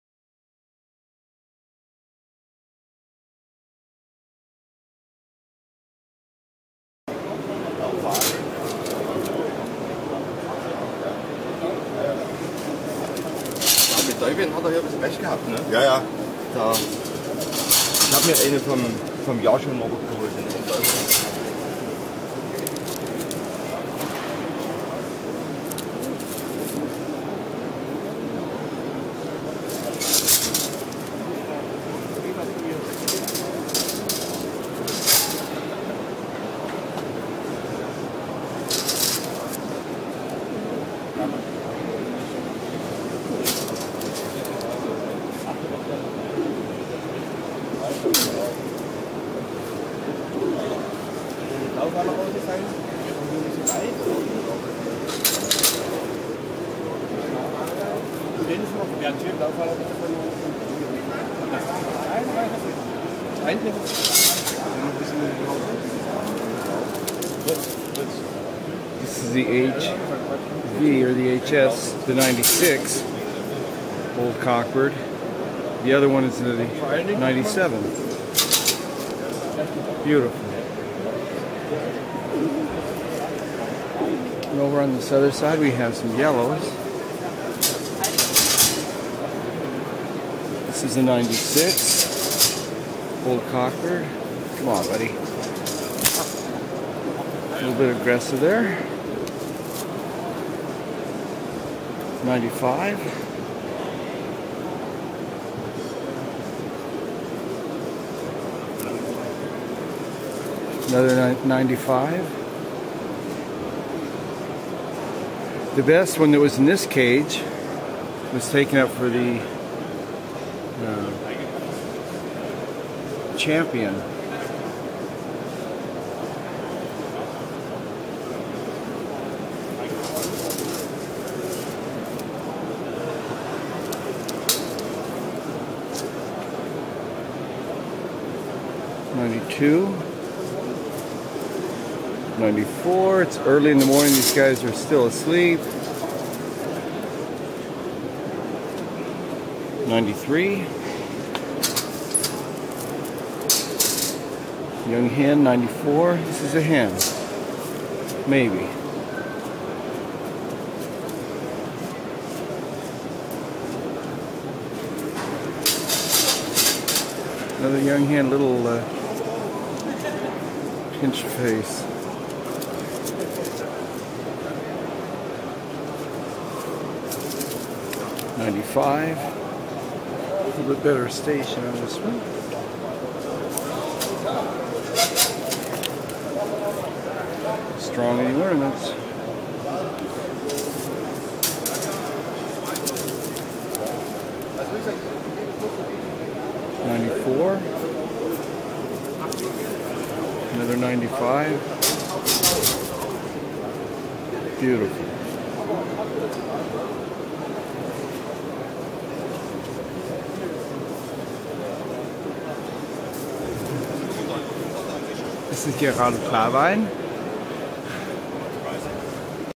Red Tiger, Yellow Tiger, Yellow Old Dutch Capuchines at Leipzig 2011
Old Dutch Capuchines at the VDT show in Leipzig in Dec. of 2011. Overall quality is very good with many birds showing wonderful body style and strength of ornaments. Tigered Capuchines are very difficult to breed. Good job here.